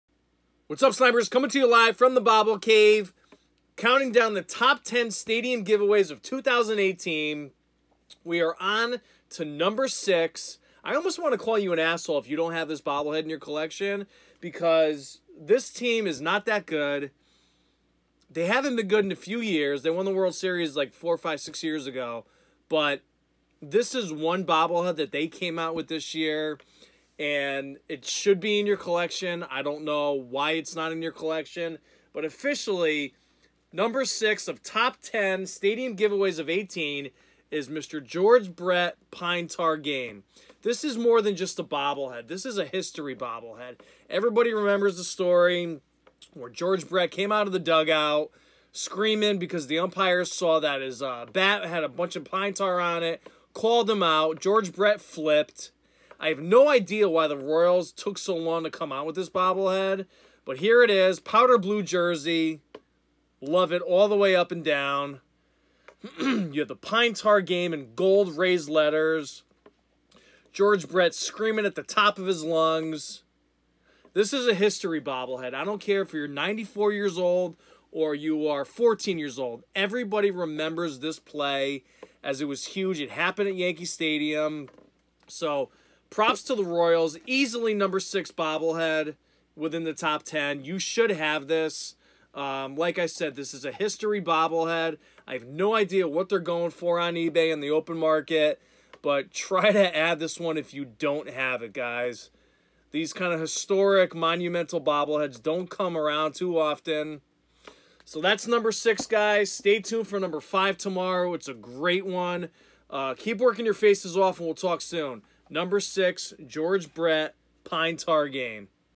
Bobble Sniper Top 10 SGA Bobbleheads of 2018 #6
In this episode, I discuss the number 6th ranked top 10 SGA bobblehead of 2018.

For additional information, head over to bobblesniper.com